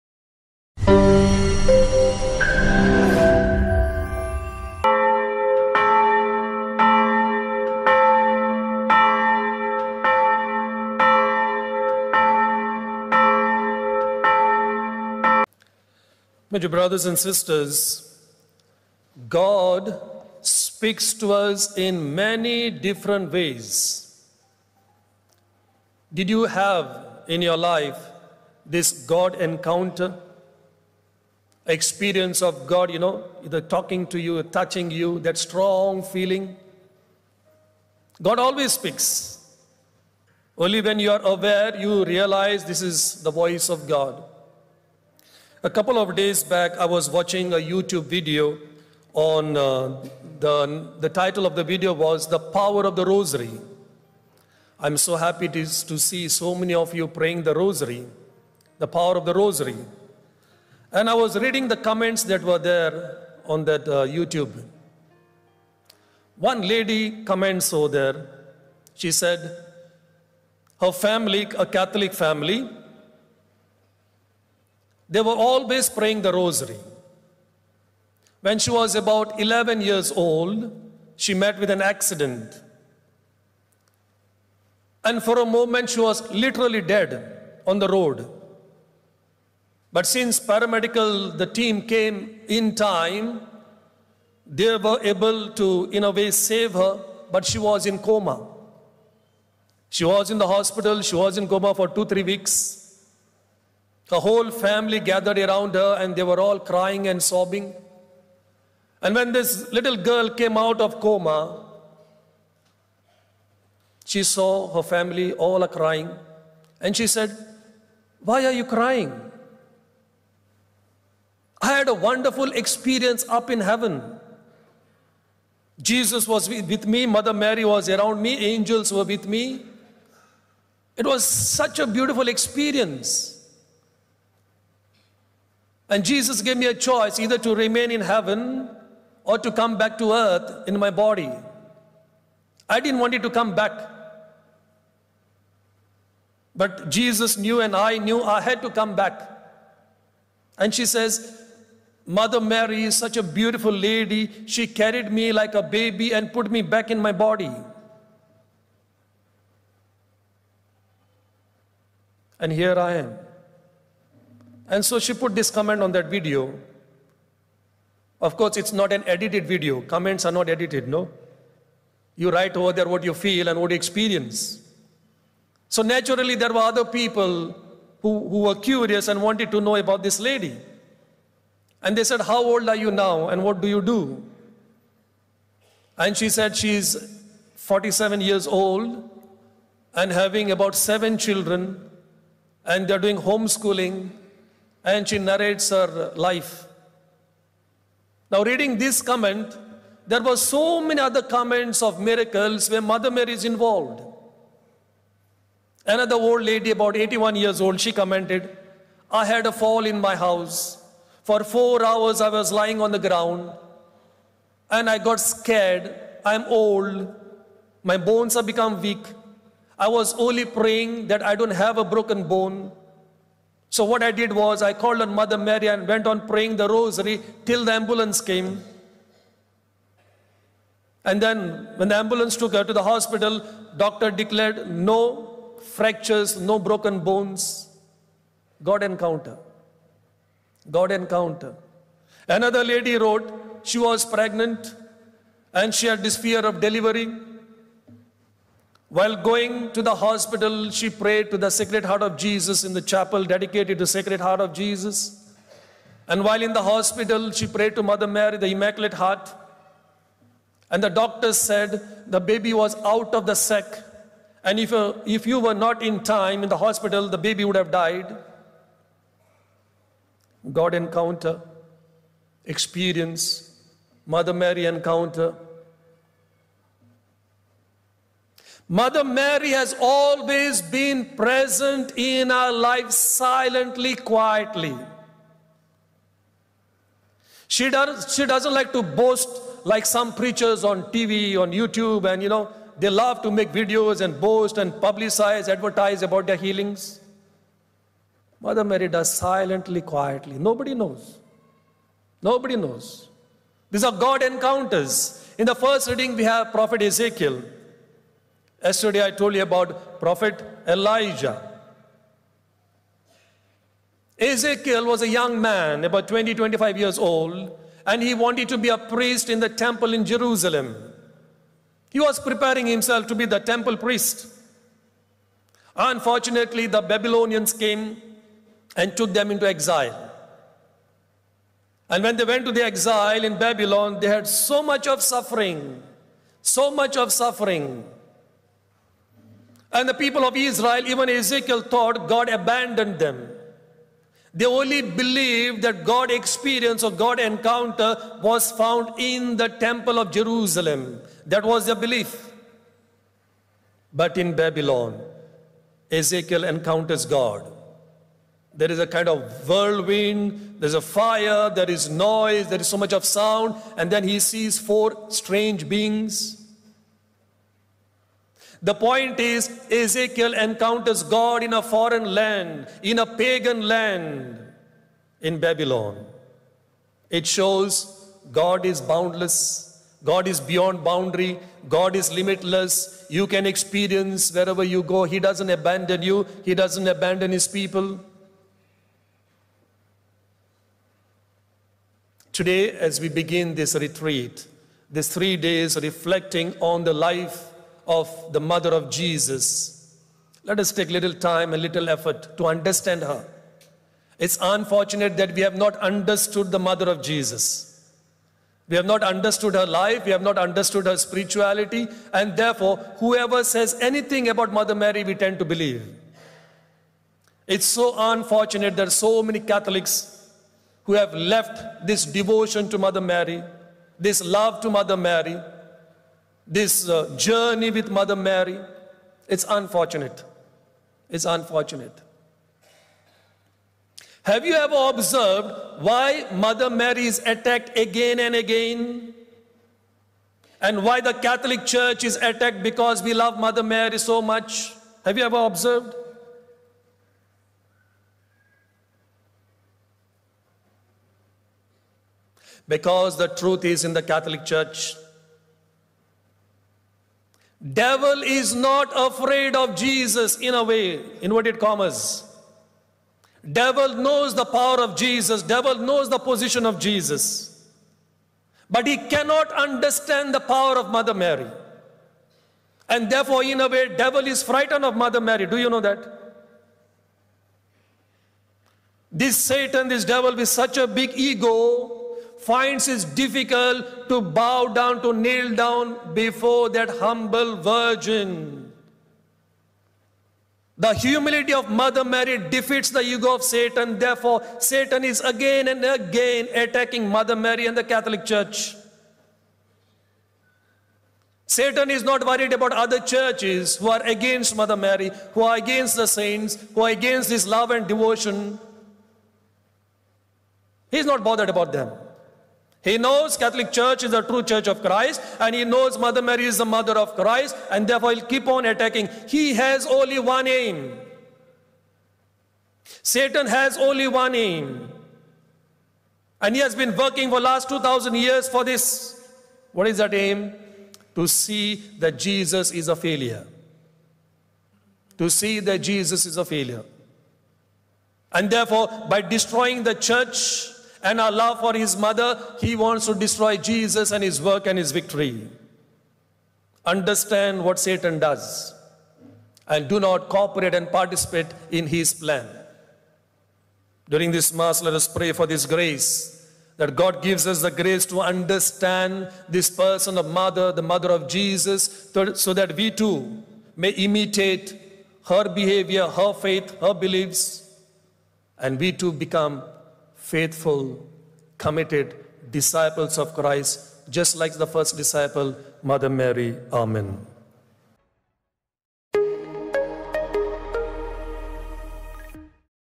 God Encounter through the Rosary # Fr Nelson Lobo OFM Cap
Why Mother Mary and the Catholic Church is attacked again and again?
The Devil cannot understand the power of Mother Mary. He is scared of Mother Mary.
Real life stories about God Encounters.
Rosary miracles.
Love for Mother Mary
Ezekiel encounters God in Babylon in a pagan land.